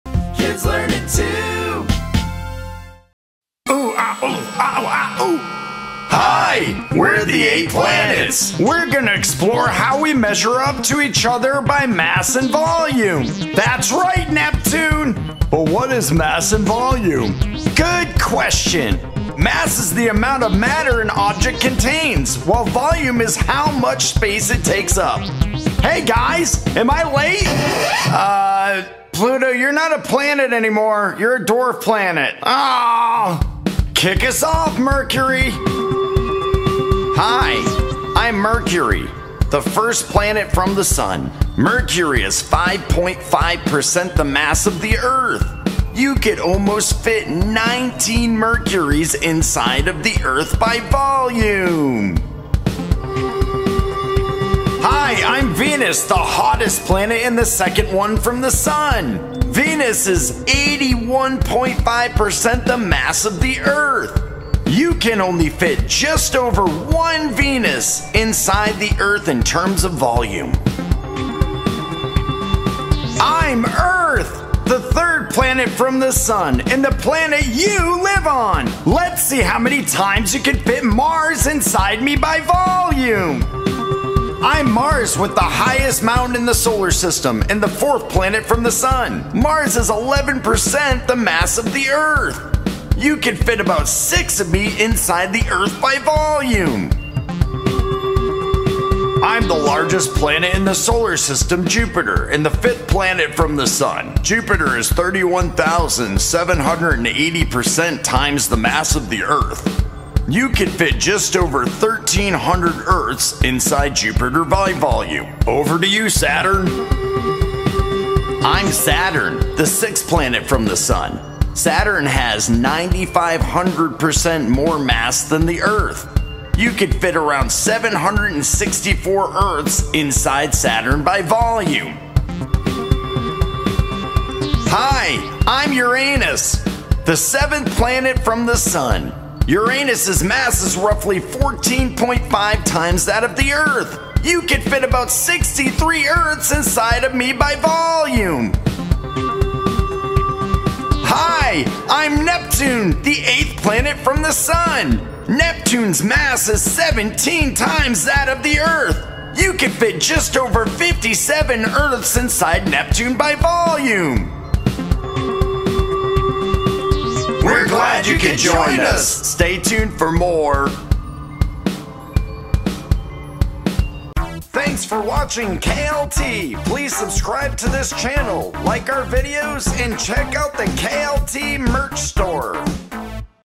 Full Solar System Size Comparison! | KLT
Let's learn ALL about the size of each part of our solar system!